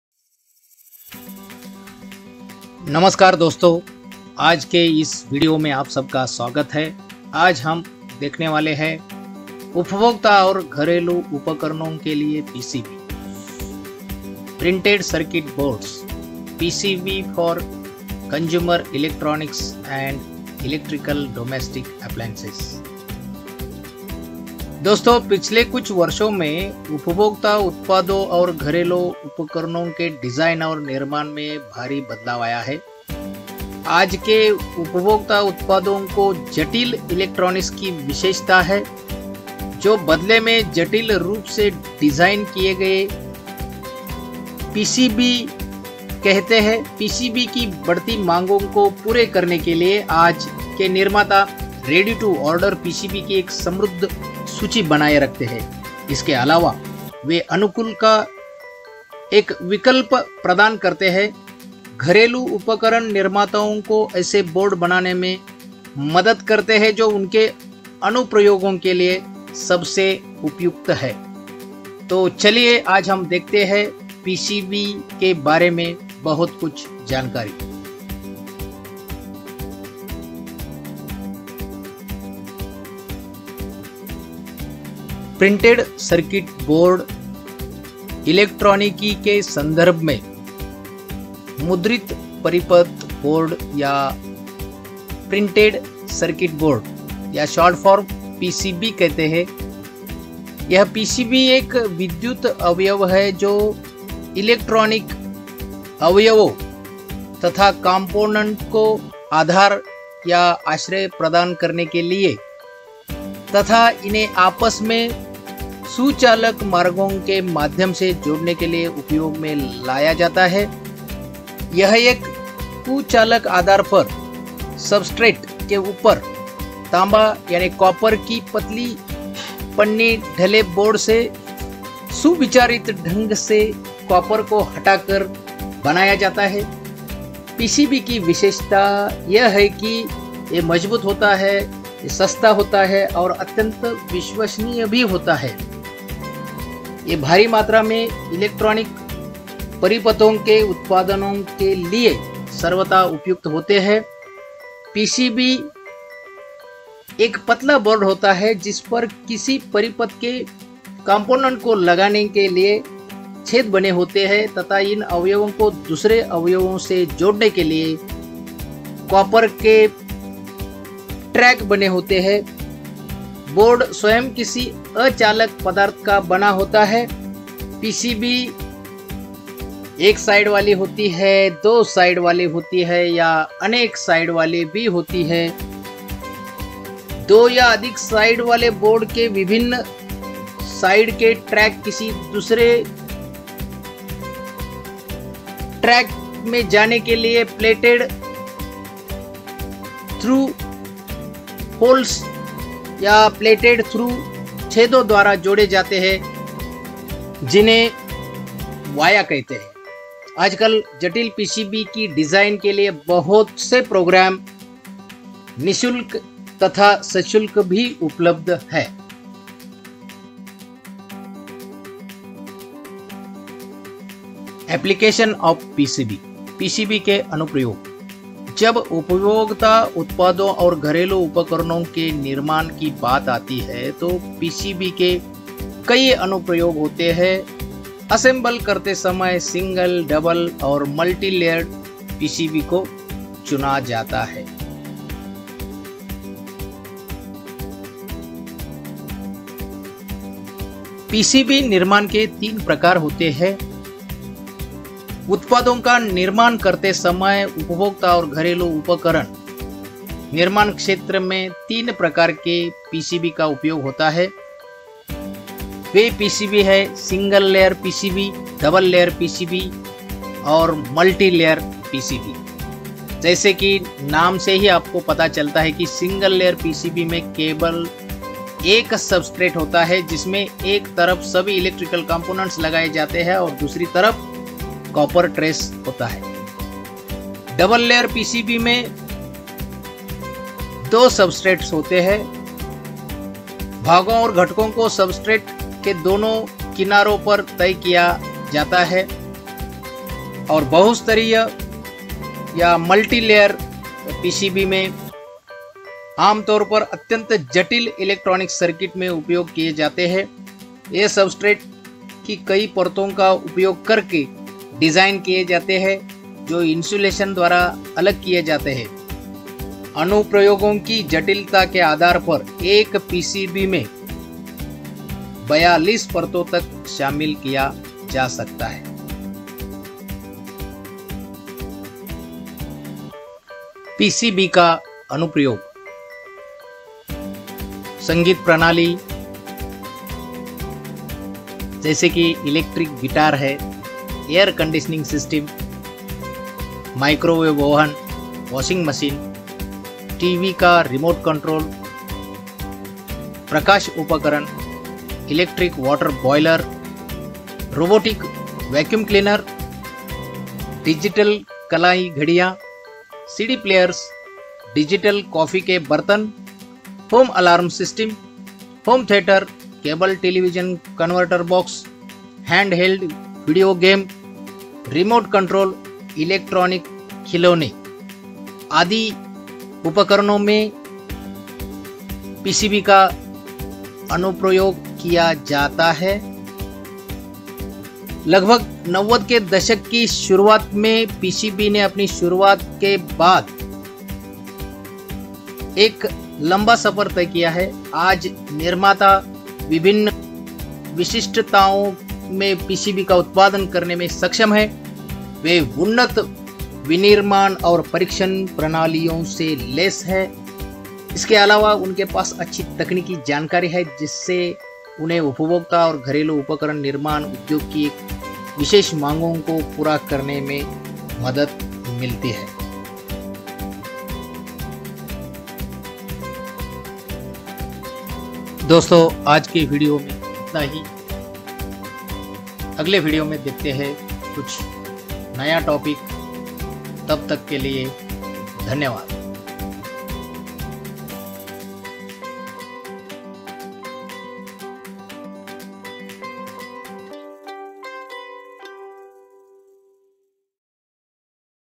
PCB PRINTED CIRCUIT BOARD USED IN ELECTRONICS मुद्रित परिपथ बोर्ड प्रिन्टेड सर्किट बोर्ड या पीसीबी
मुद्रित परिपथ बोर्ड एक पतला बोर्ड होता है जिस पर किसी परिपथ के अवयवों को लगाने के लिए छेद बने होते हैं तथा इन अवयवों को दूसरे अवयवों से जोड़ने के लिए कॉपर के पथ (ट्रैक) बने होते हैं। बोर्ड स्वयं किसी अचालक पदार्थ का बना होता है। पीसीबी एक-साइड वाली हो सकती है, दो साइड वाली हो सकती है या अनेकों साइड वाली भी होती है। दो या अधिक साइड वाले बोर्ड के विभिन्न तलों (साइड) के ट्रैक किसी दूसरे तल में जाने के लिए प्लेटेड-थ्रू-छेदों द्वारा जोड़े जाते हैं जिन्हें वाया (via) कहते हैं।

आजकल जटिल पीसीबी की डिजाइन के लिए बहुत से प्रोग्राम (निःशुल्क या सशुल्क) उपलब्ध हैं।
PCB PRINTED CIRCUIT BOARD USED IN ELECTRONICS मुद्रित परिपथ बोर्ड प्रिन्टेड सर्किट बोर्ड या